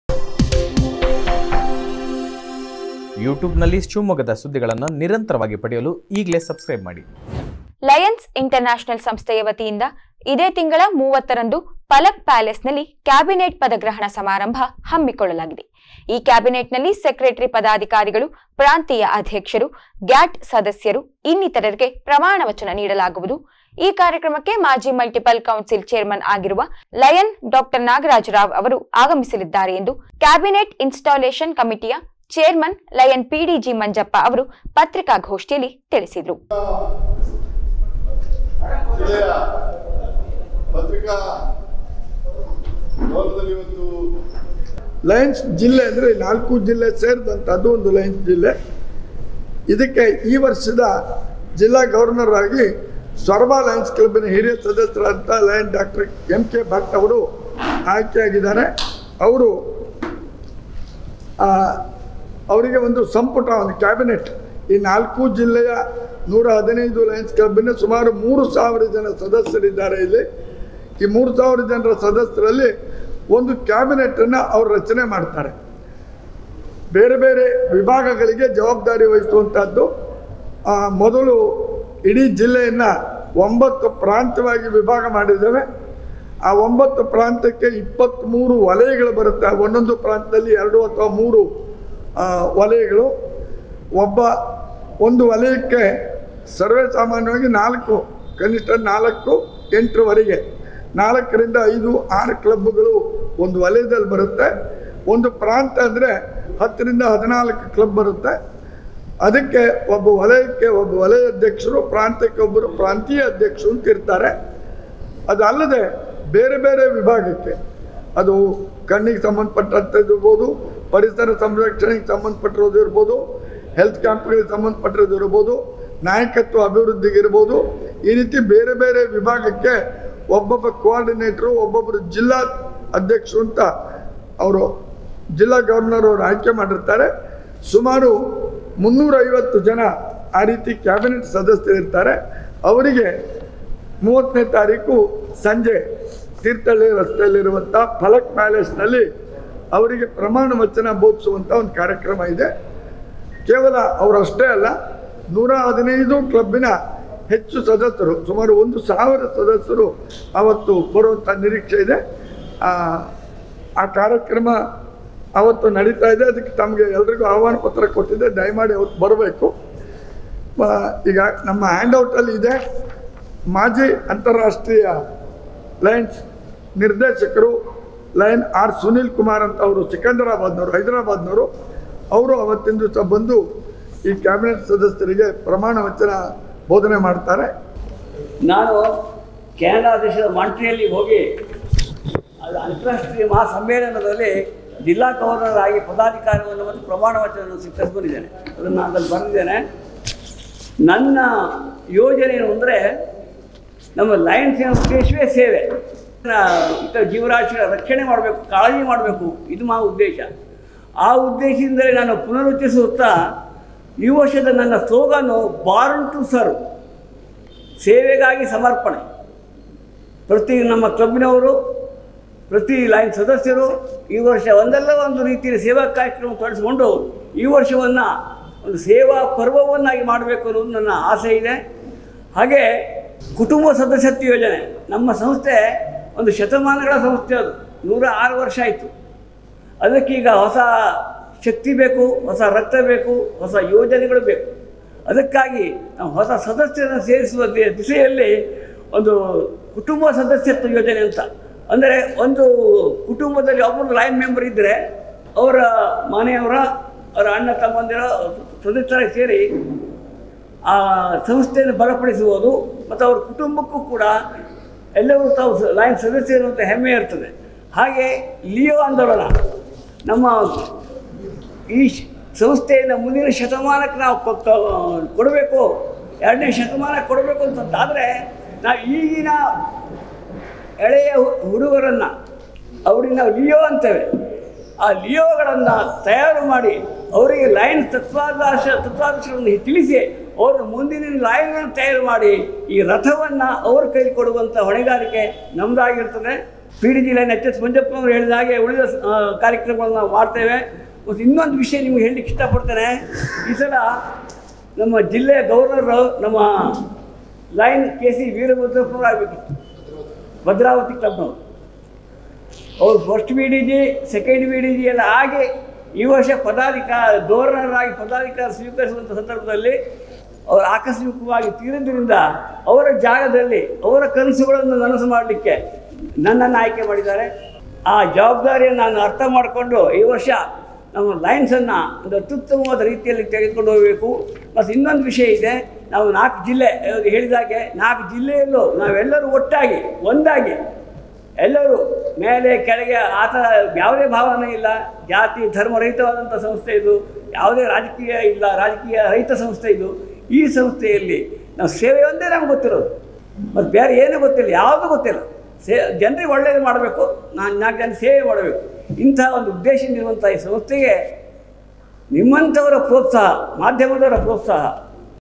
ಜೇಸಿ ಪದಗ್ರಹಣ | Councilchaitman Lion International
ಲಯನ್ಸ್‌ ಇಂಟರ್ನ್ಯಾಷನಲ್‌ ಸಂಸ್ಥೆಯ ವತಿಯಿಂದ ಇದೇ ತಿಂಗಳ 30 ರಂದು ಫಲಕ್‌ ಪ್ಯಾಲೇಸ್‌ ನಲ್ಲಿ ಕ್ಯಾಬಿನೇಟ್‌ ಪದಗ್ರಹಣ ಸಮಾರಂಭ ಹಮ್ಮಿಕೊಳ್ಳಲಾಗಿದೆ. ಈ ಕ್ಯಾಬಿನೇಟ್ನಲ್ಲಿ ಸೆಕ್ರೆಟರಿ ಪದಾದಿಕಾರಿಗಳು, ಪ್ರಾಂತೀಯ ಅದ್ಯಕ್ಷರು, ಗ್ಯಾಟ್‌ ಸದಸ್ಯರು ಇನ್ನಿತರರಿಗೆ ಪ್ರಮಾಣವಚನ ನೀಡಲಾಗುವುದು. ಈ ಕಾರ್ಯಕ್ರಮಕ್ಕೆ ಮಾಜಿ ಮಲ್ಟಿಪಲ್‌ ಕೌನ್ಸಿಲ್‌ ಛೇರ್ಮನ್‌ ಆಗಿರುವ ಲಯನ್‌ ಡಾ ನಾಗರಾಜ್‌ ರಾವ್‌ ಅವರು ಆಗಮಿಸಲಿದ್ದಾರೆ ಎಂದು ಕ್ಯಾಬಿನೇಟ್‌ ಇನ್ಸ್ಟಲೇಷನ್‌ ಕಮಿಟಿಯ ಛೇರ್ಮನ್‌ ಲಯನ್‌ ಪಿಡಿಜಿ ಮಂಜಪ್ಪ ಅವರು ಪತ್ರಿಕಾಗೋಷ್ಟಿಯಲ್ಲಿ ತಿಳಿಸಿದರು.
shivamogga